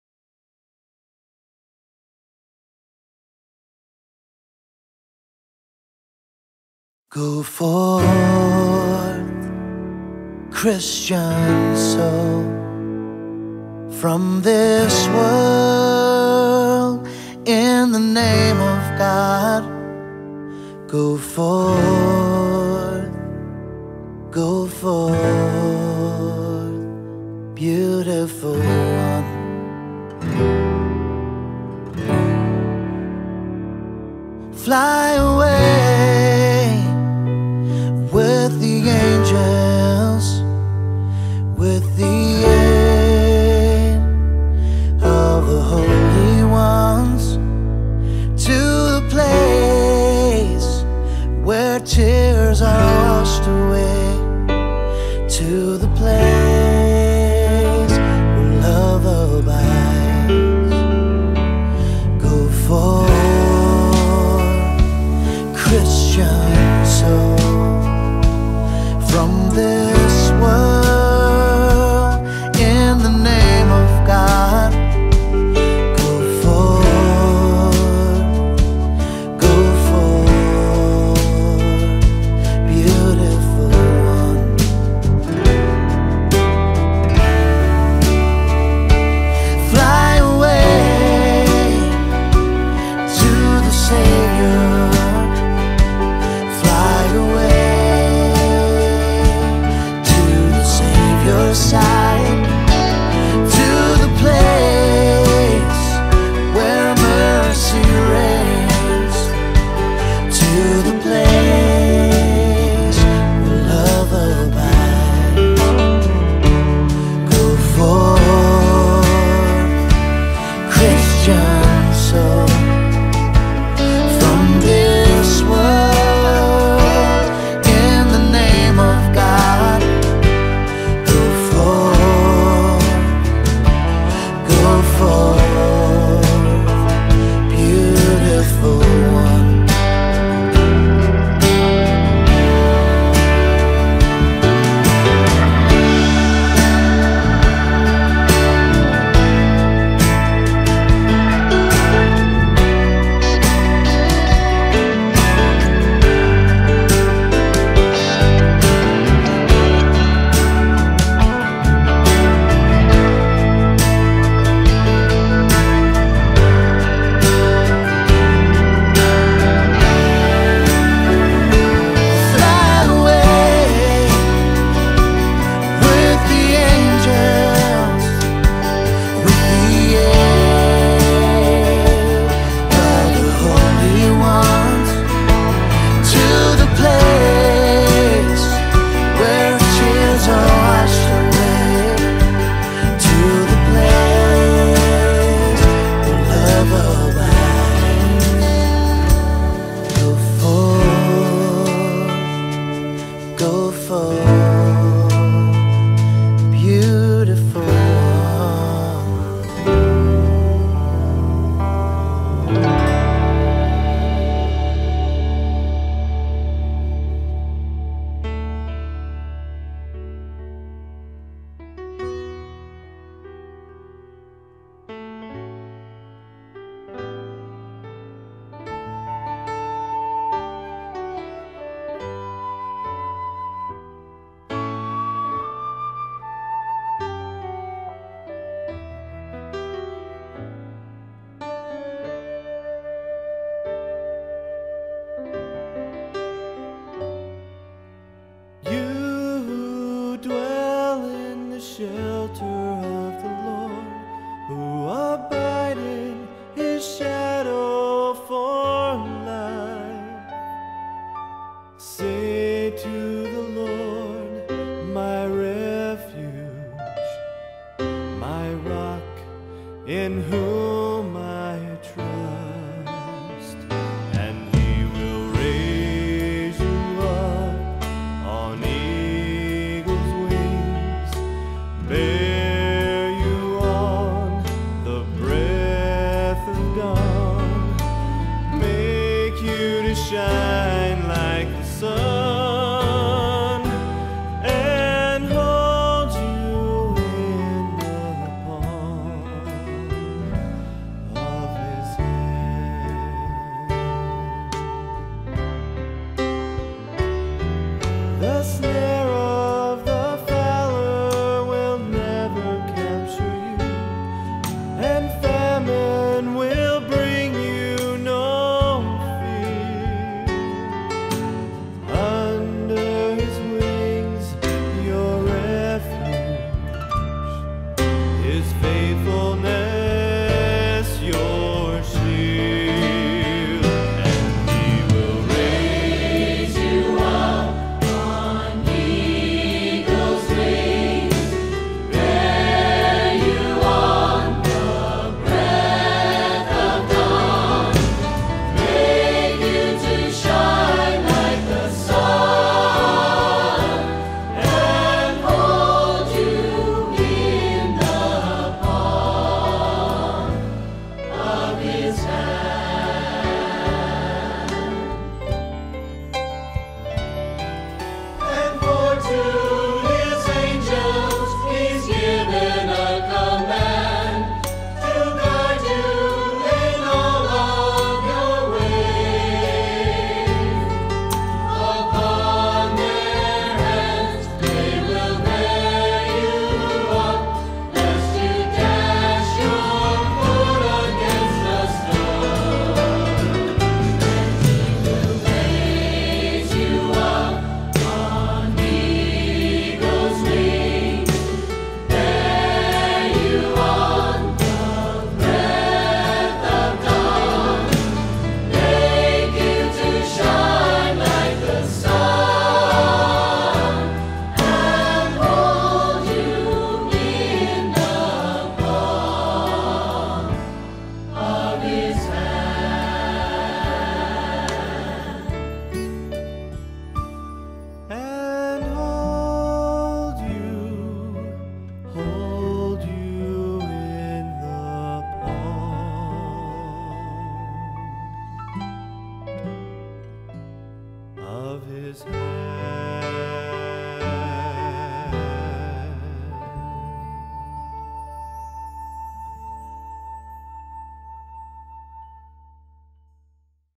All Souls Day Remembrance Video 2018
Video of our loved ones who have passed on to eternal Life.